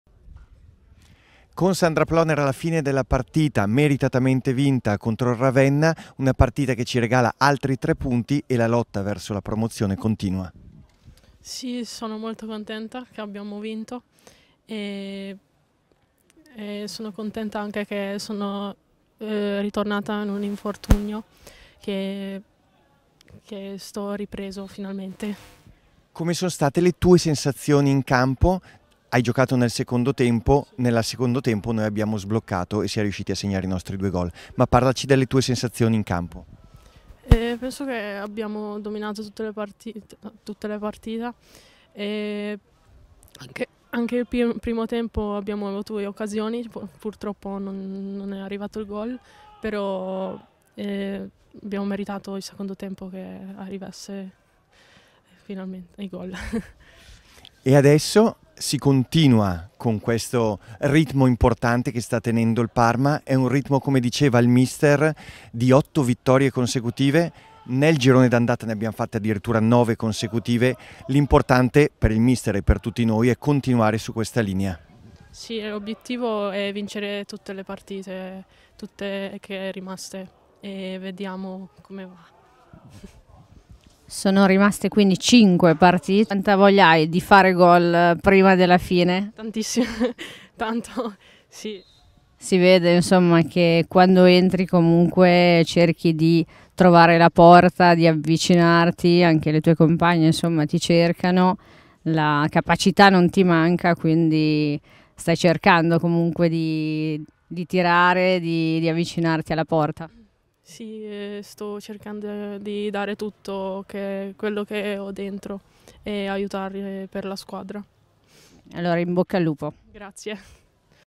Post Match: Ploner dopo Parma-Ravenna
Ascolta le parole di Szandra Ploner dopo la vittoria in campionato contro il Ravenna